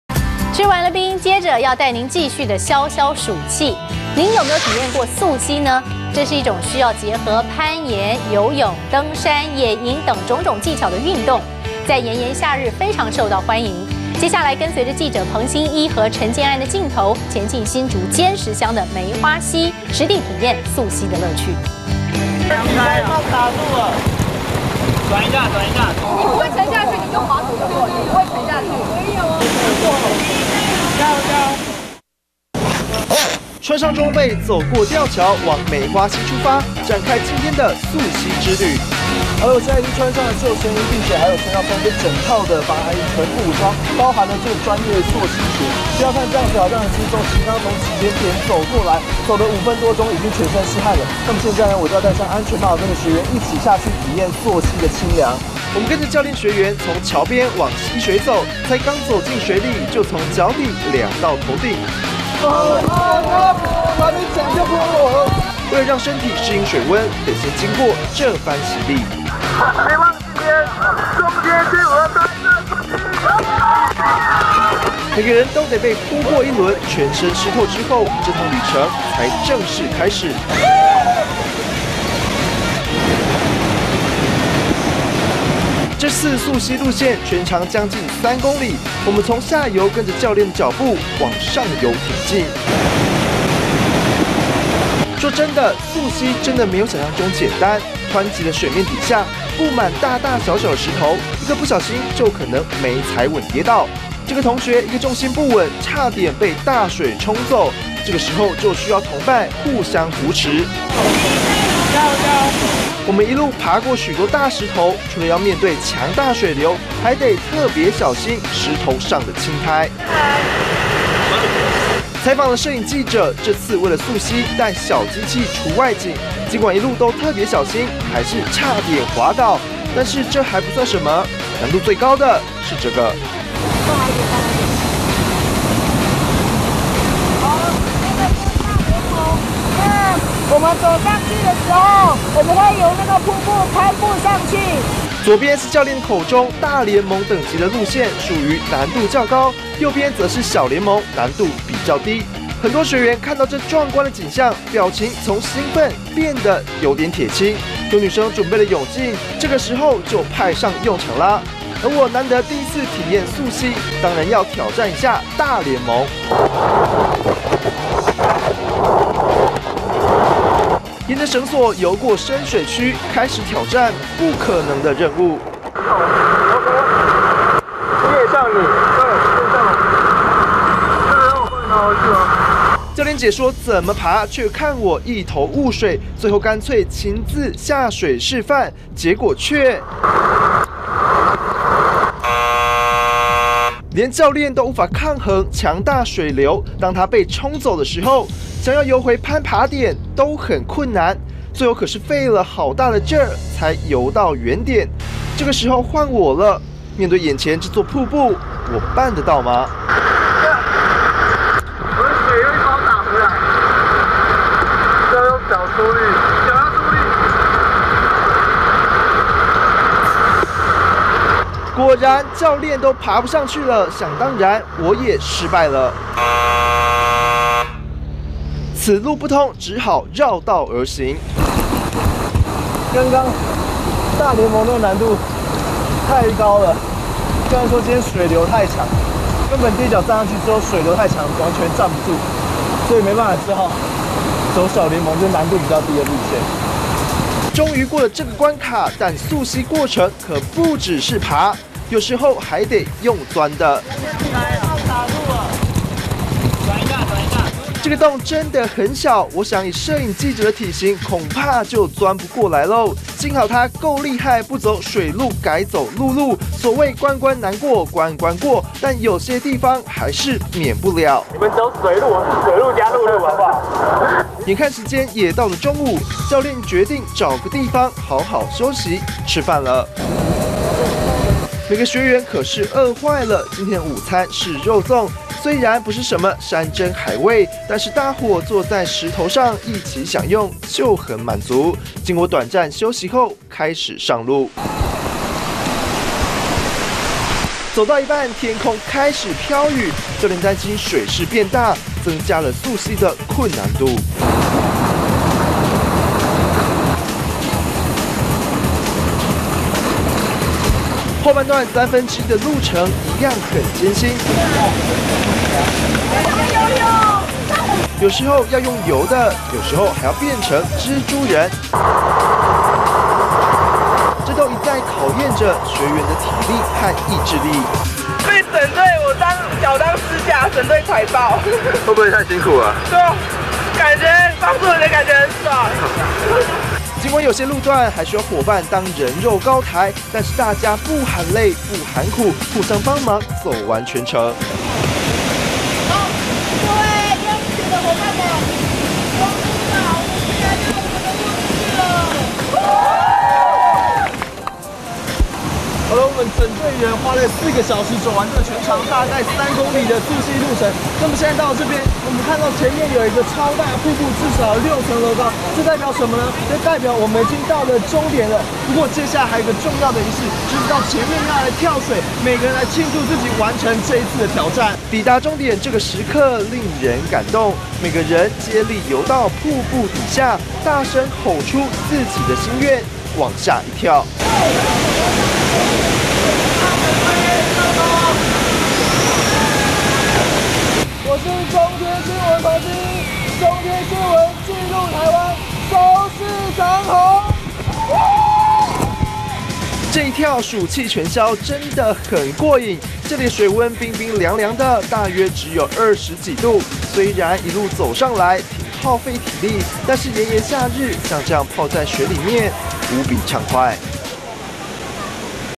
2013.09.15紀錄台灣 炎下著裝溯溪去 沁涼溪水消暑氣
體驗過溯溪嗎？這是一種需要結合攀岩，游泳，登山，野營等技巧的運動，在炎炎夏日，相當受到歡迎．跟著記者彭心一陳建安的鏡頭，前進新竹尖石鄉的梅花溪，實際體驗溯溪的樂趣。